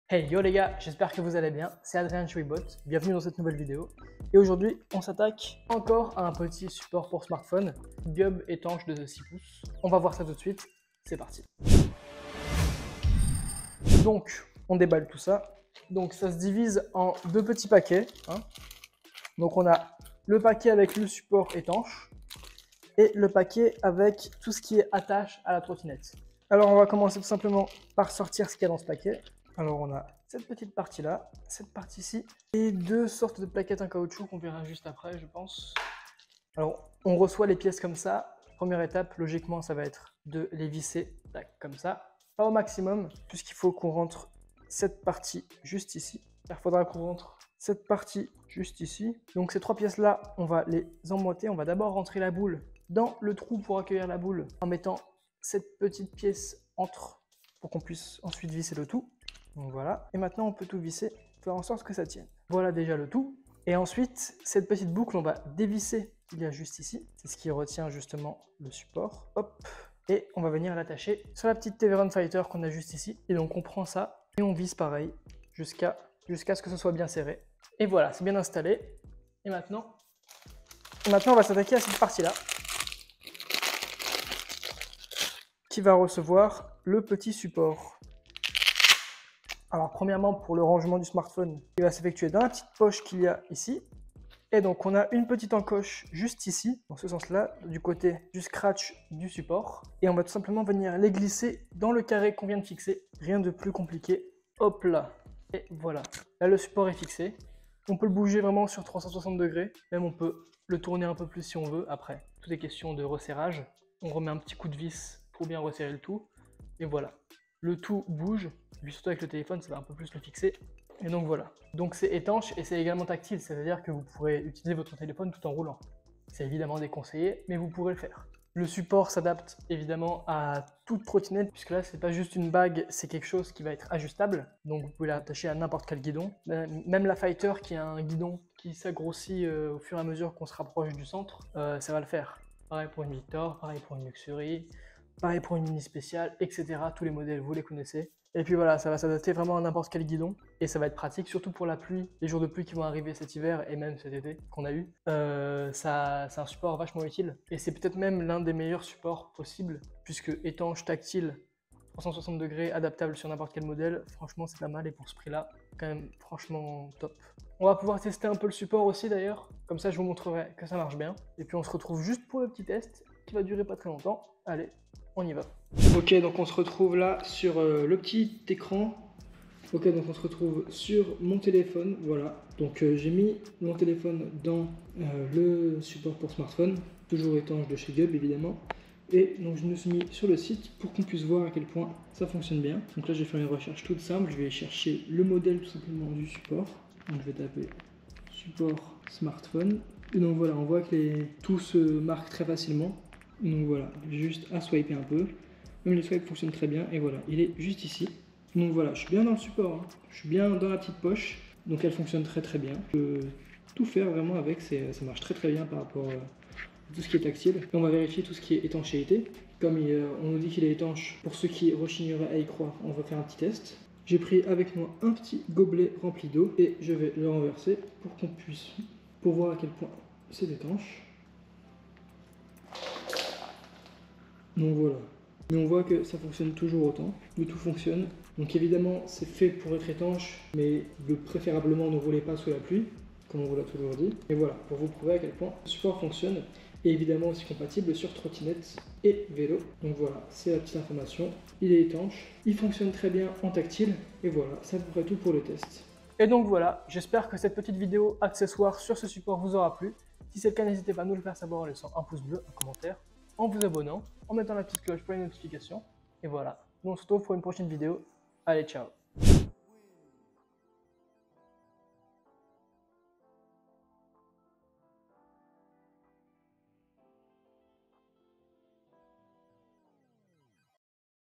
SUPPORT SMARTPHONE ETANCHE GUB 919 POUR TROTTINETTE : LE SUPPORT TÉLÉPHONE WATERPROOF !
Aujourd'hui on se penche sur un support pour smartphone GUB, étanche et super fin !

Bon visionnage !!!

🎁​Téléchargez notre guide gratuit et multipliez par 2 la durée de vie de votre trottinette

📍 Retrouvez-nous sur :
➡️Notre site web :

Chapitres :
Intro : 00:00
Présentation : 00:14
Démonstration : 2:56
Outro : 4:17